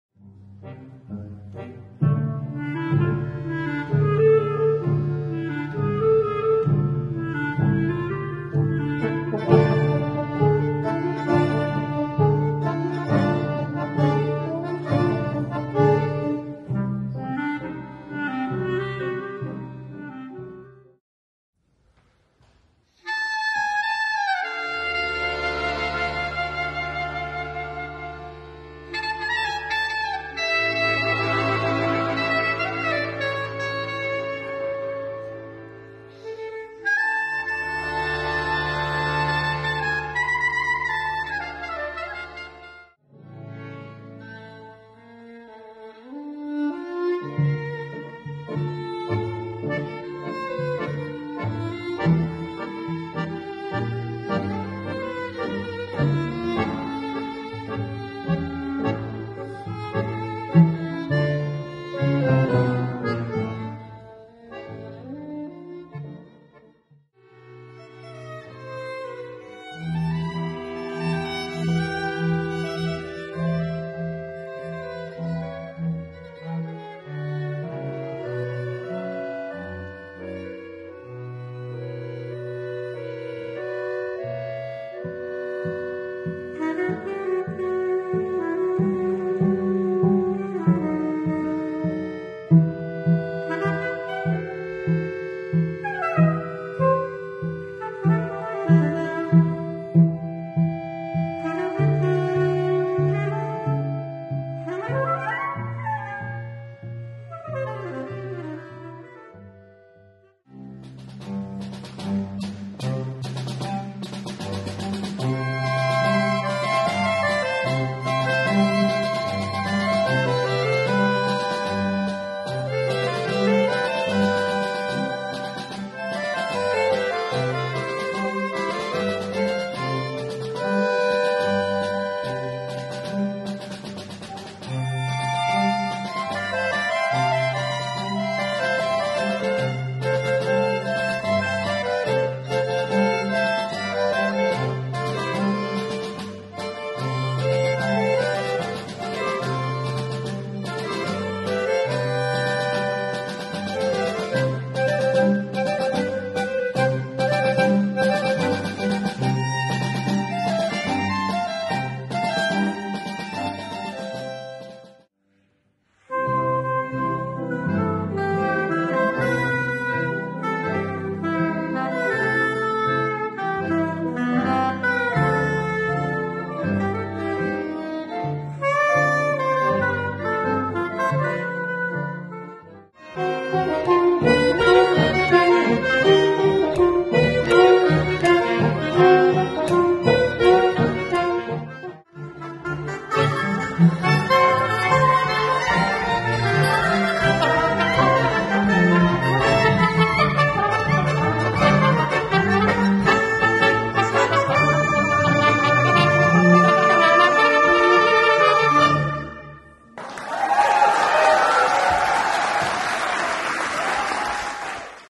The music of Marc Chagall's paintings
Timo King and his Orchestra performing scores, which inspired Marc Chagall 2015